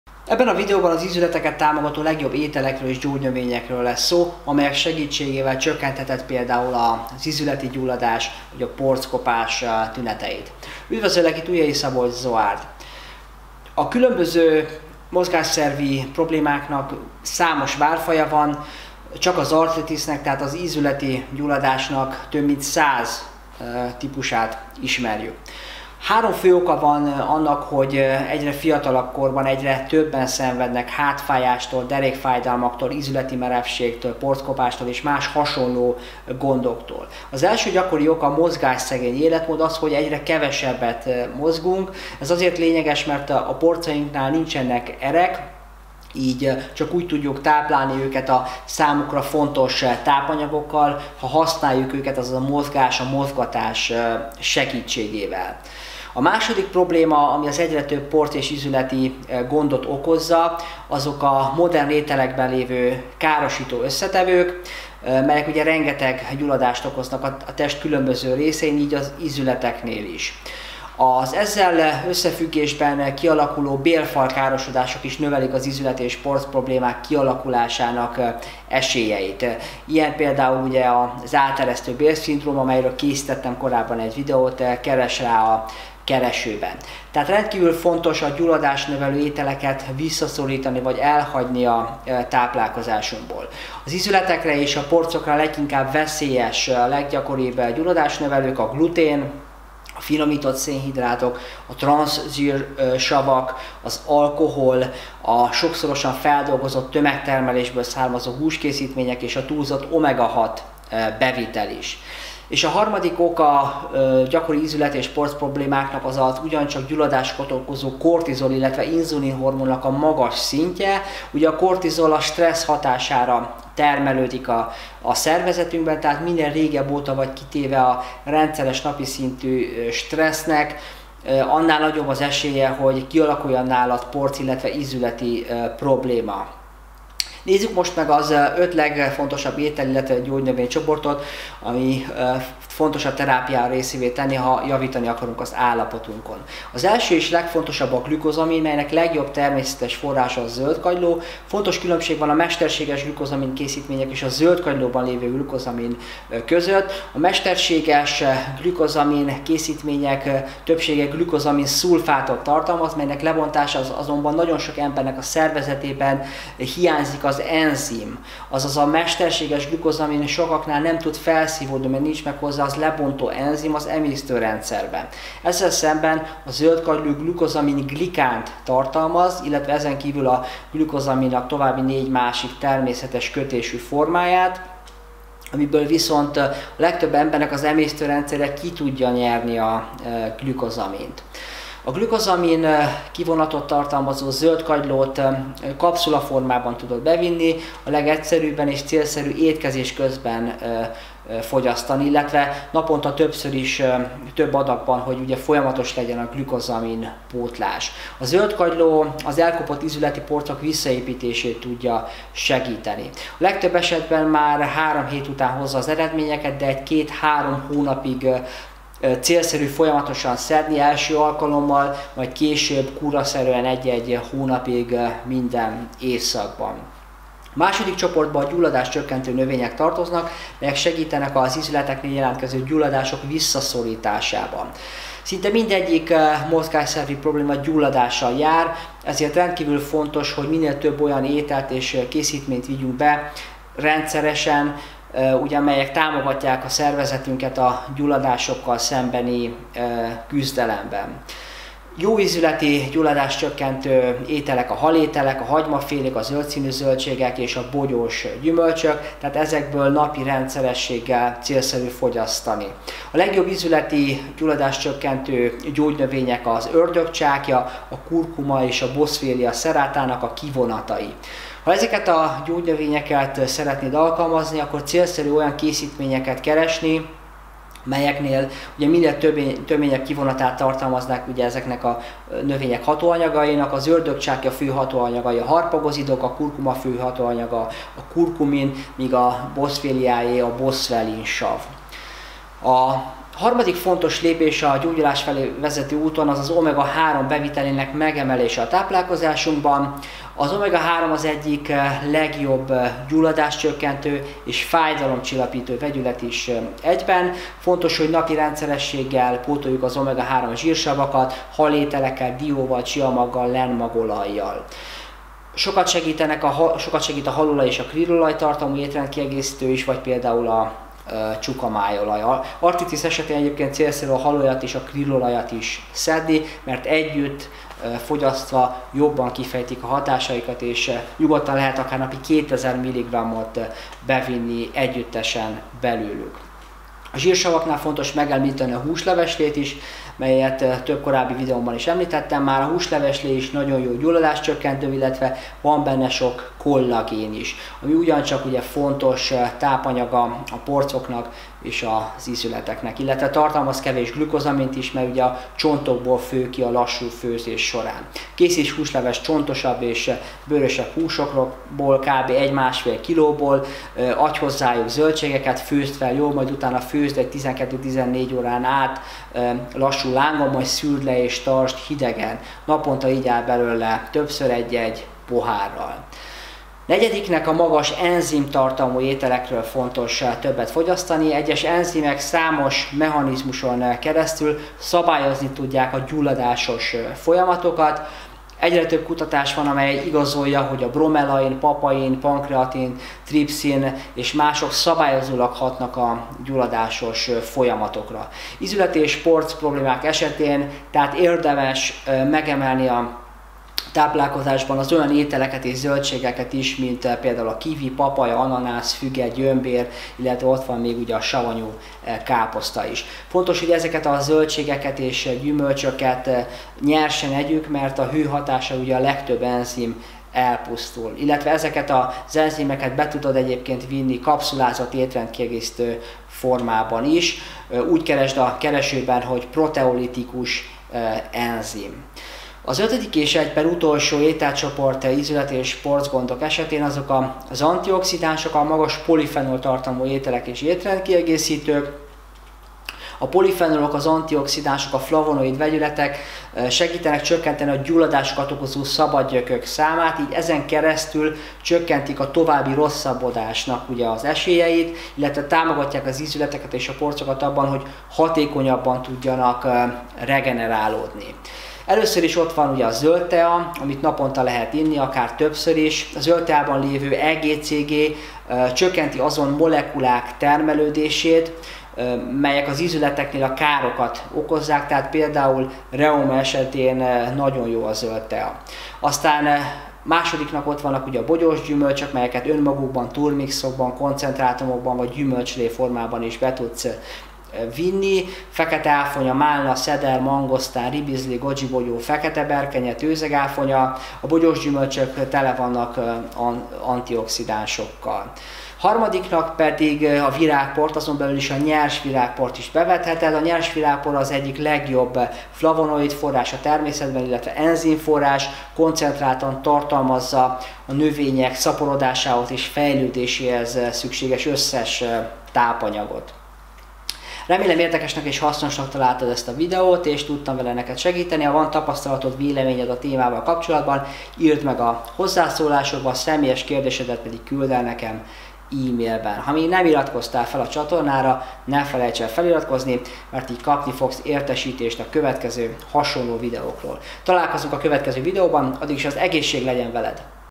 Porckopás, Arthritis esetén 5 regeneráló étel-csoport
Mitől romlik az ízületek és a porcok állapota? Mi áll a reuma, az arthritis, az ízületi kopás hátterében? Ételek, gyógynövények és hatóanyagok, melyek segíthetnek abban, hogy megszűntessük a hátfájást, a térdfájdalmat, a derékfájást és más mozgásszervi problémát.

Az osteoarthritis kezelésére javasolt növények és tápanyagok: (kollagén, boswellia, kurkumin).